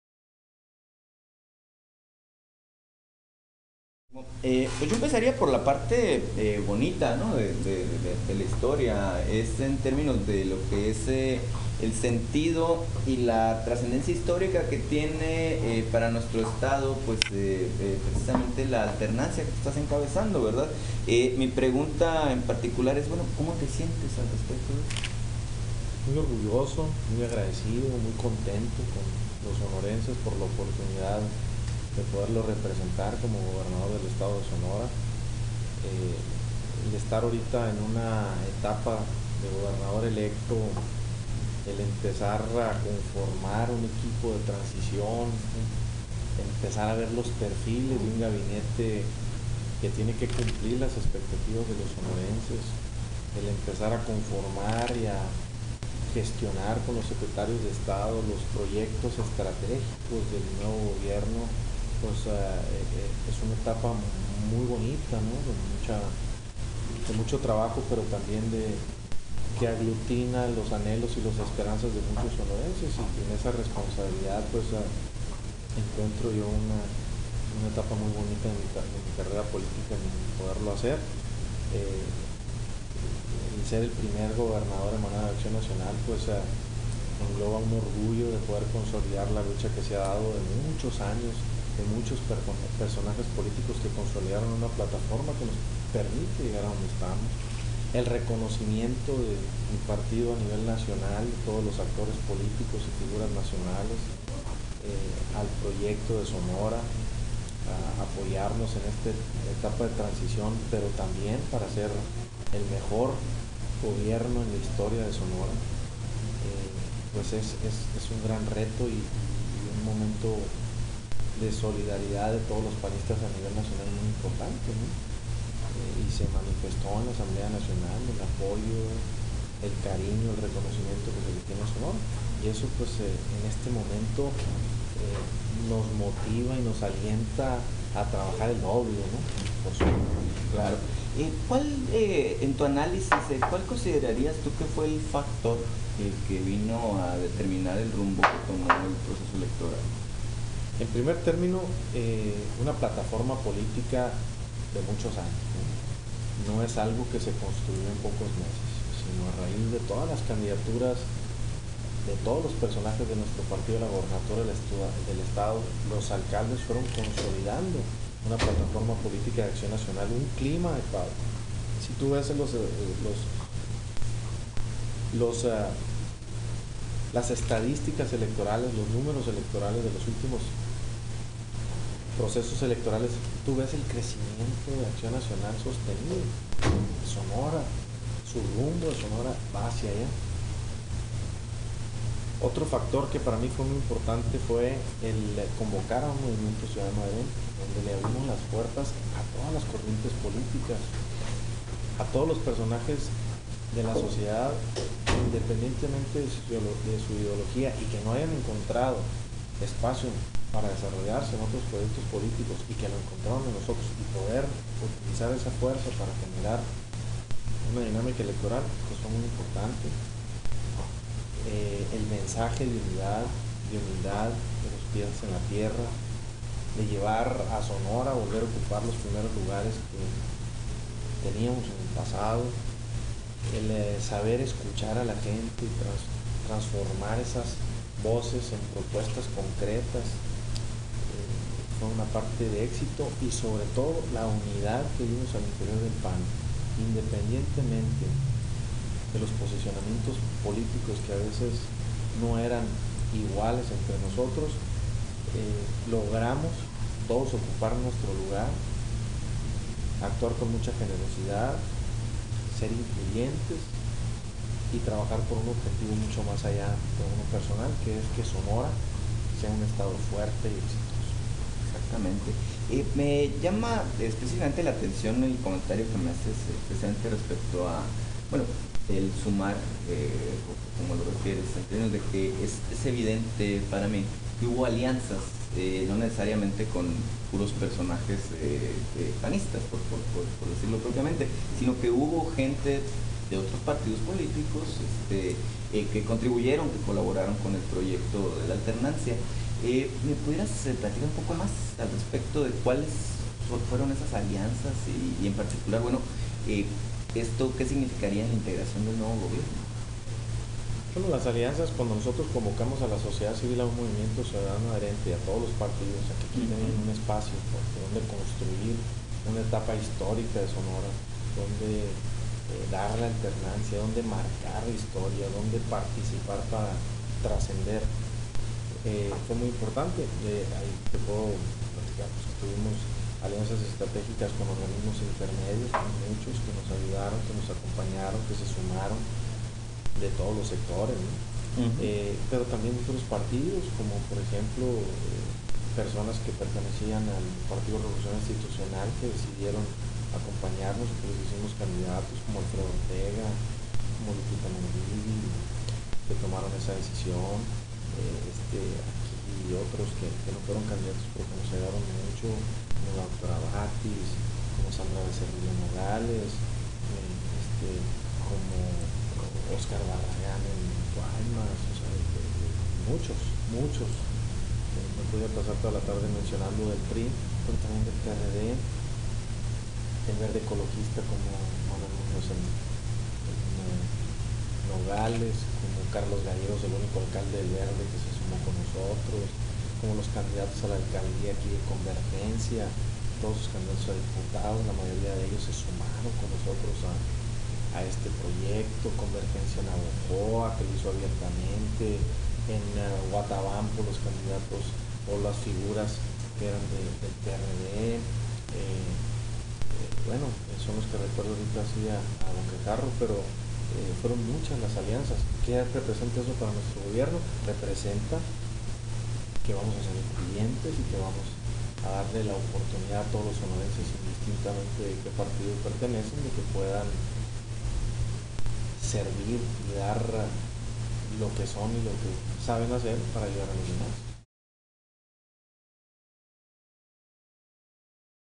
Entrevista Guillermo Padrés... "Fueron muchas las alianzas" (Primera Parte)
Entrevista a Guillermo Padrés, Gobernador Electo de Sonora. Por: Demian Duarte García.